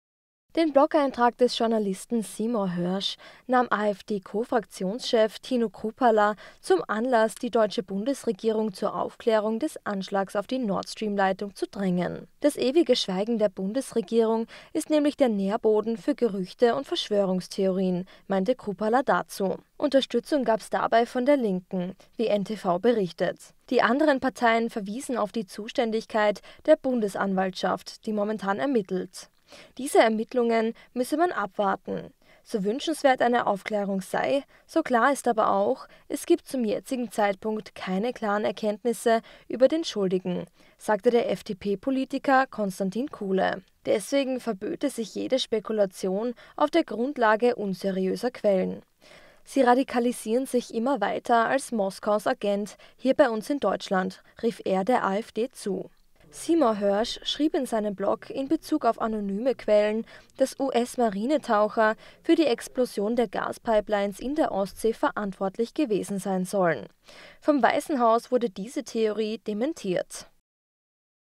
Streit im Bundestag wegen Nord-Stream-Explosion
AfD und Linke verlangten im Deutschen Bundestag Aufklärung in der Nord-Stream-Explosion. Das lange Schweigen der Bundesregierung würde nur zu Spekulationen und Verschwörungstheorien führen, hieß es dabei, nachdem der US-Journalist Seymour Hersh US-Marinetaucher für die Explosion verantwortlich gemacht hat.